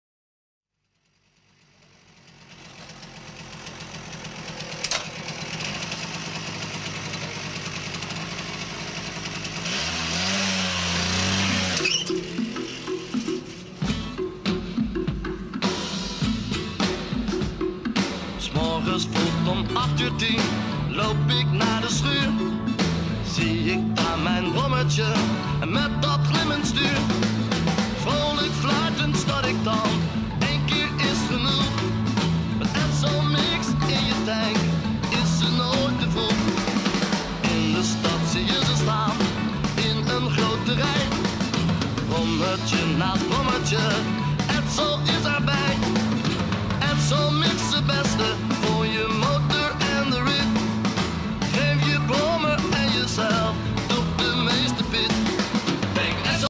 Nieuwe brommer tijdens BCL bedrijfsbezoek Huisman Tweewielers.
Grote verrassing, tijdens een bedrijfsbezoek aan Huisman Tweewielers kreeg ik namens de leden (dus niet de BCL zelf omdat ik onafhankelijk wil blijven ) van de Business Club Leiden een nieuwe brommer aangeboden.